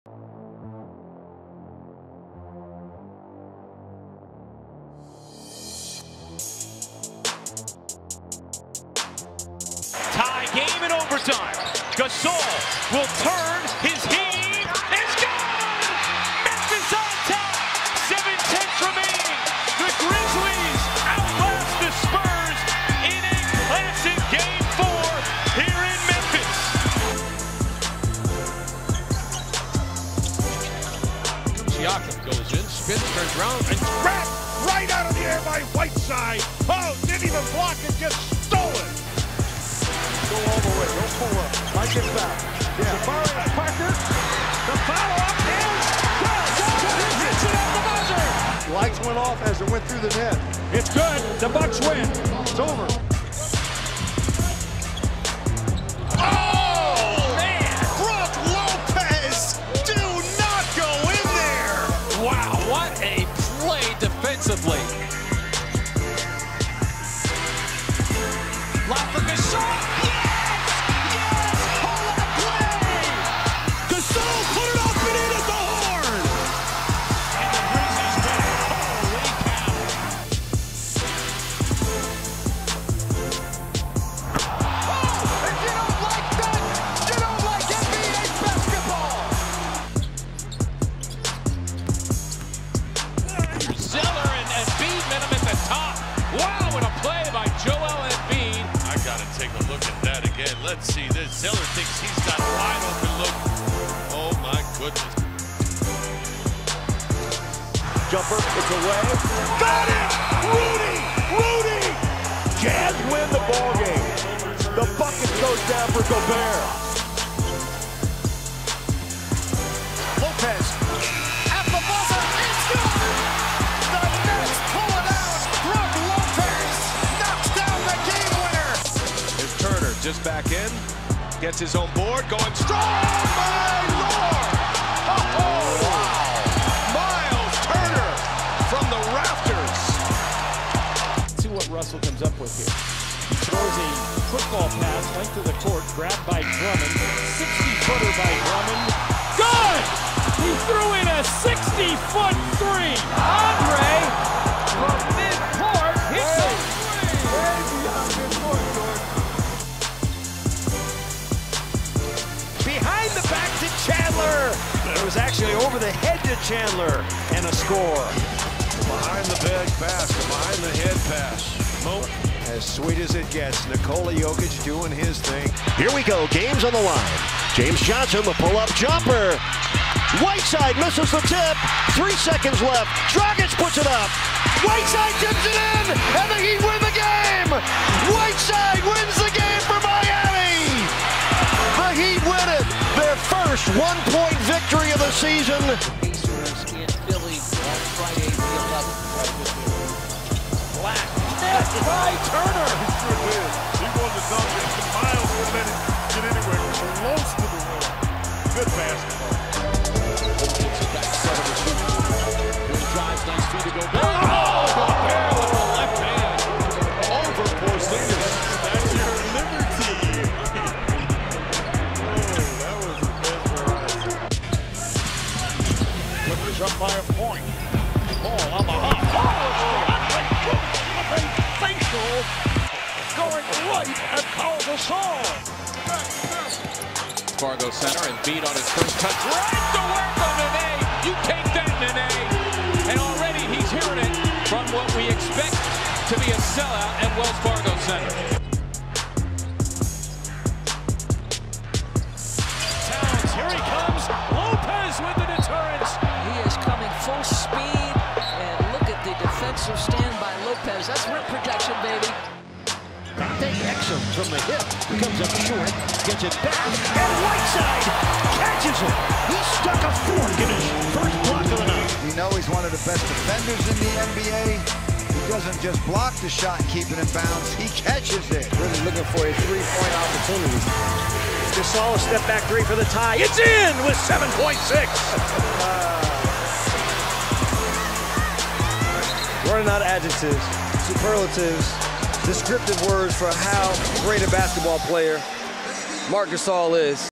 The Best Plays From Centers: 2017 NBA Season
Relive the top plays of centers from the 2017 NBA season, featuring Mark Gasol, Hassan Whiteside and Joel Embiid!